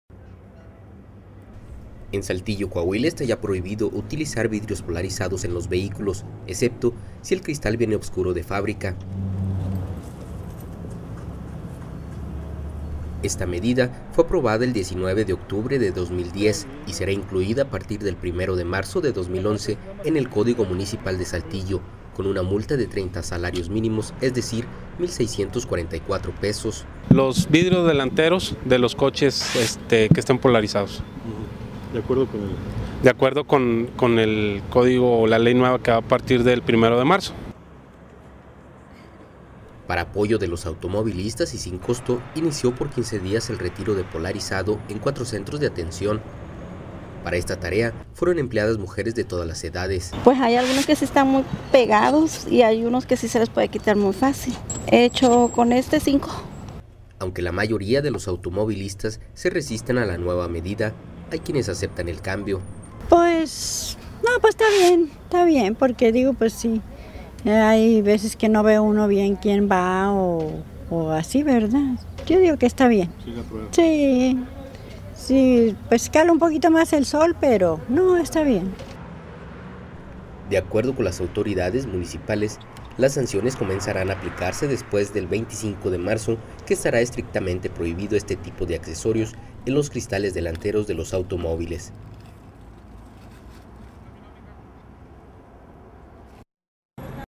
INICIA RETIRO POLARIZADO
En Saltillo, Coahuila esta ya prohibido utilizar vidrios polarizados en los vehículos, excepto, si el cristal  viene obscuro de fábrica.

Esta medida fue aprobada el 19 de octubre de 2010 y será incluida  a partir del primero de marzo de 2011, en el Código Municipal de Saltillo, con una multa de 30 salarios mínimos, es decir, mil 644 pesos.